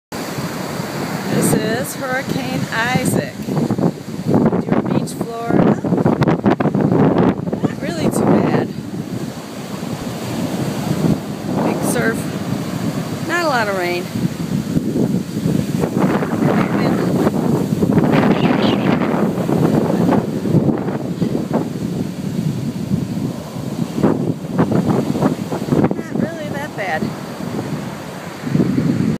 Isaac on Madeira beach fl 1
Video of surf during hurricane isaac